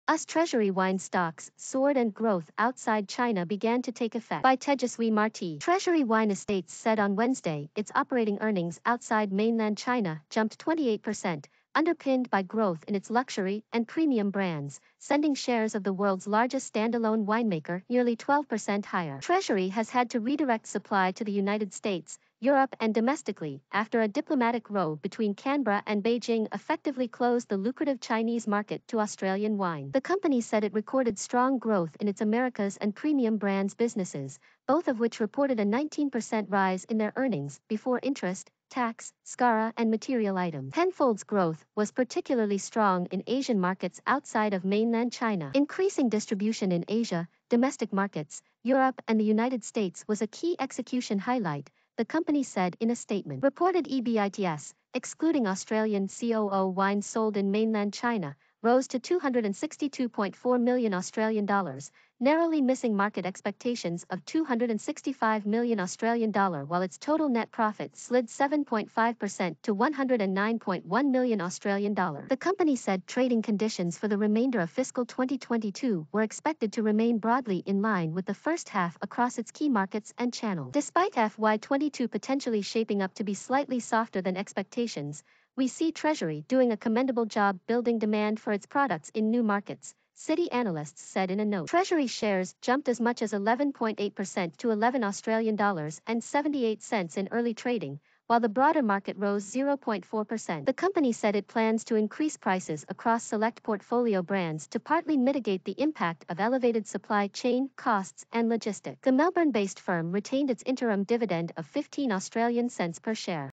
Us Treasury wine stocks soared and growth outside China began to take effect
Europe and domestically after a diplomatic row between Canberra and Beijing effectively closed the lucrative Chinese market to Australian wine. The company said it recorded strong growth in its Americas and premium brands businesses Us Treasury wine stocks soared and growth outside China began to take effect By Tejaswi Marthi -Treasury Wine Estates said on Wednesday its operating earnings outside mainland China jumped 28% " Citi analysts said in a note. Treasury shares jumped as much as 11.8% to A$11.78 in early trading narrowly missing market expectations of A$265 million while its total net profit slid 7.5% to A$109.1 million. The company said trading conditions for the remainder of fiscal 2022 were expected to remain broadly in line with the first half across its key markets and channels. "Despite FY22 potentially shaping up to be slightly softer than expectations while the broader market rose 0.4%. The company said it plans to increase prices across select portfolio brands to partly mitigate the impact of elevated supply chain costs and logistics. The Melbourne-based firm retained its interim dividend of 15 Australian cents per share. sending shares of the world's largest standalone winemaker nearly 12% higher. Treasury has had to re-direct supply to the United States